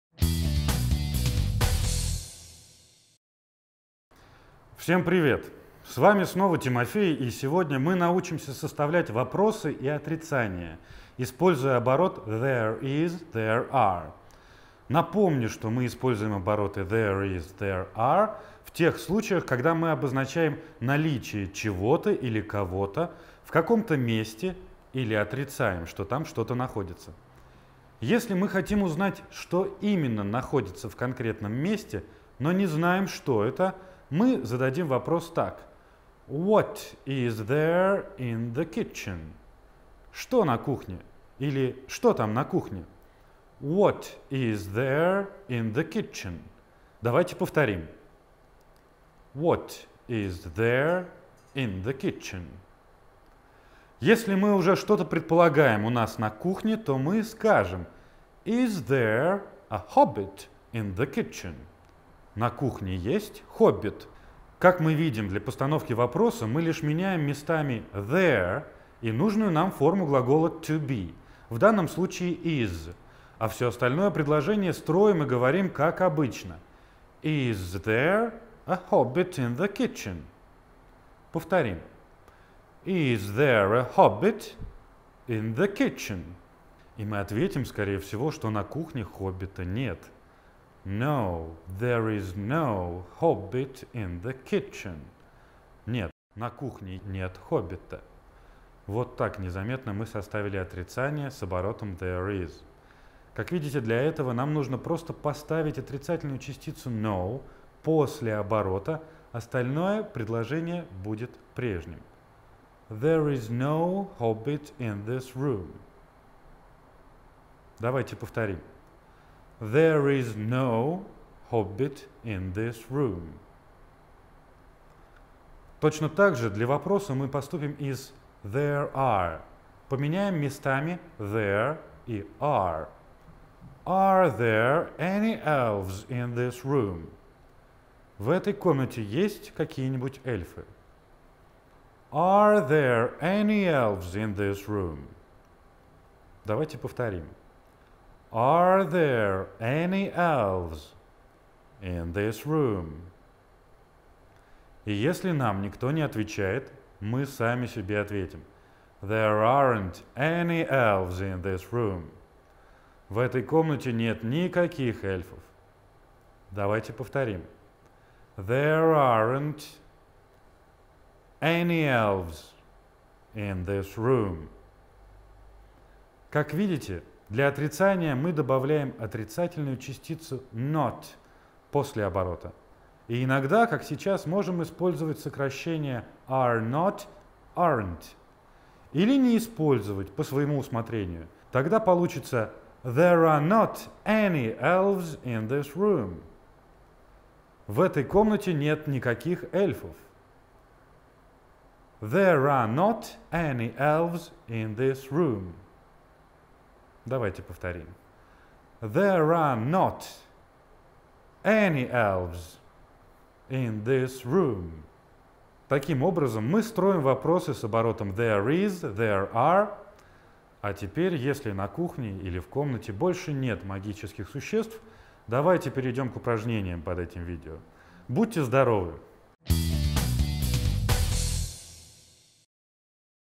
Английская грамматика: Вопросы и отрицания с THERE IS/THERE ARE
Этот урок посвящён правилам построения отрицательных и вопросительных предложений с оборотом there is/there are.